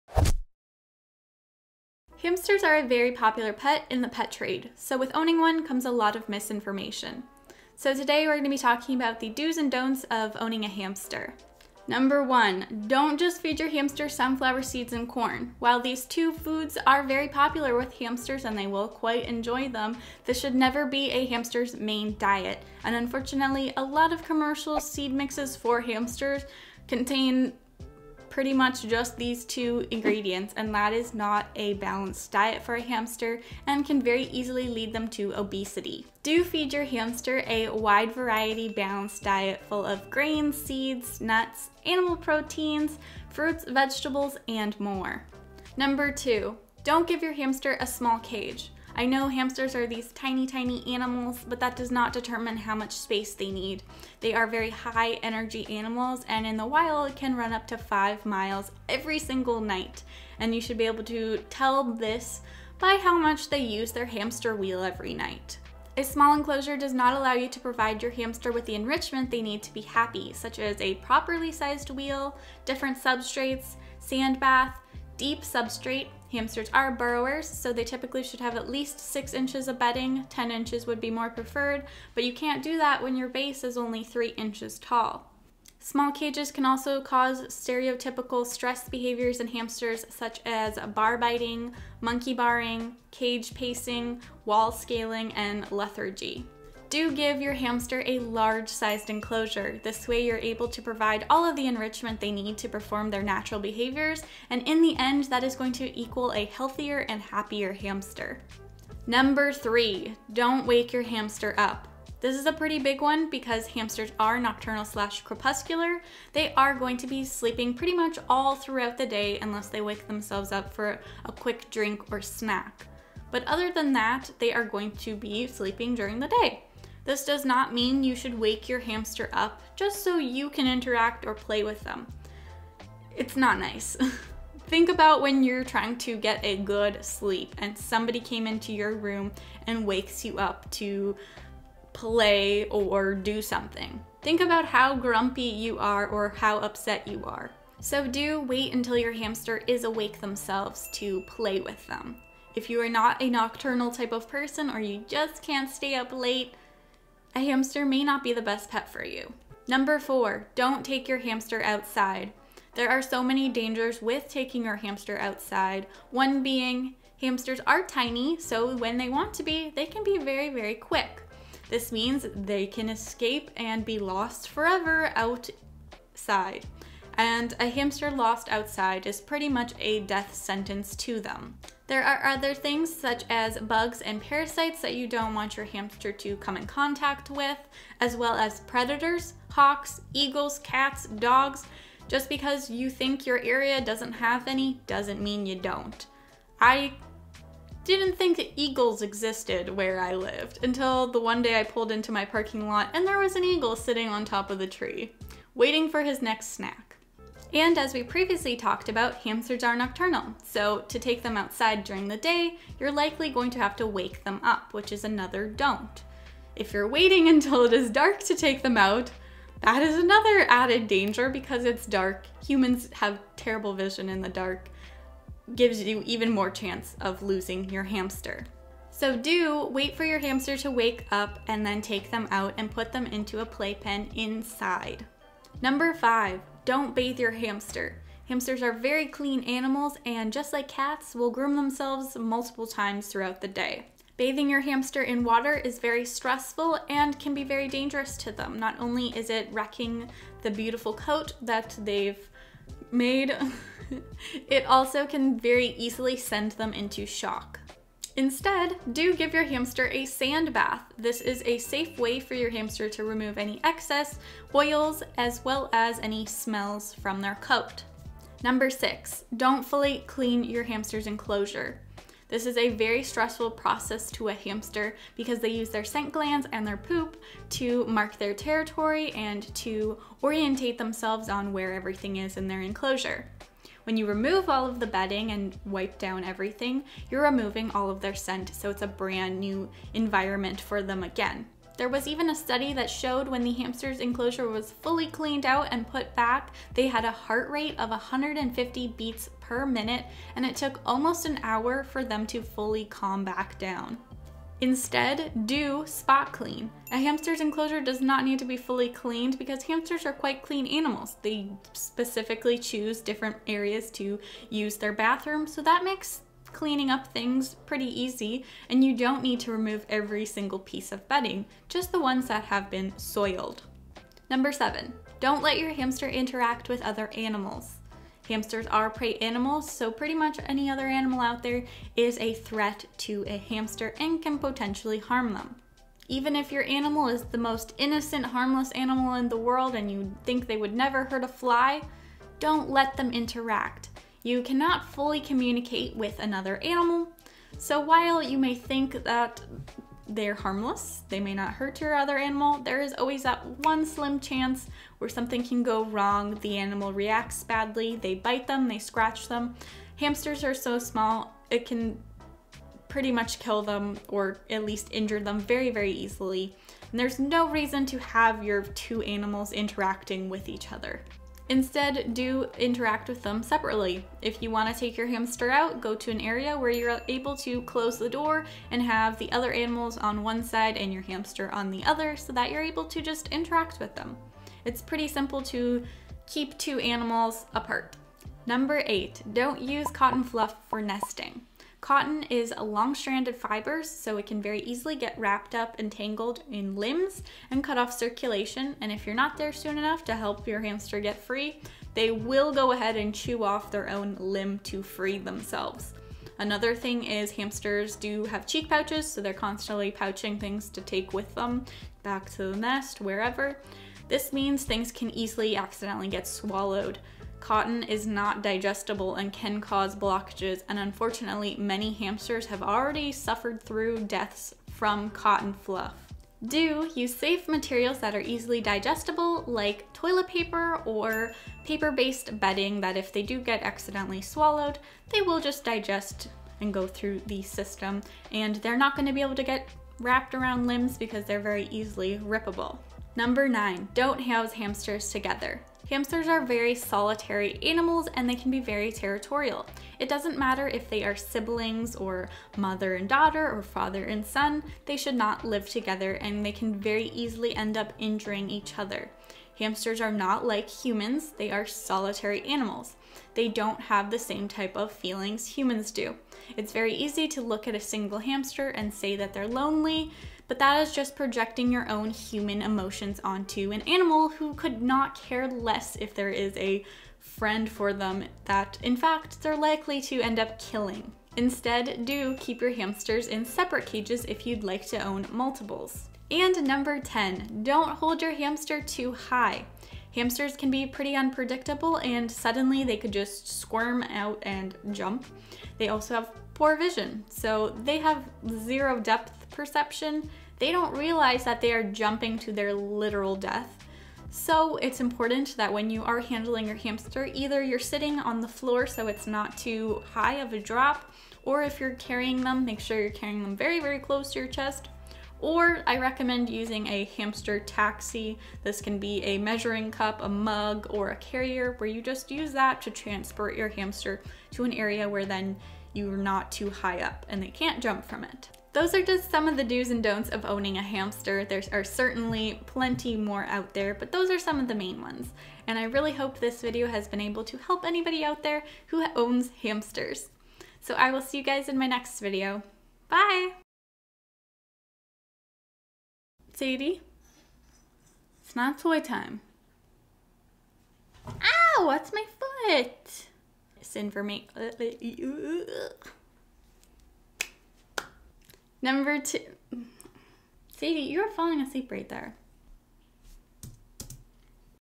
Hamster Dos & Don'ts
While many assume because hamsters are considered "beginner pets" they should be fairly easy to care for. but are they? Today we talk about the Dos and Don'ts of owning a hamster!

Cameras: Lumix GH5, Canon 70D & Canon G7X Mark II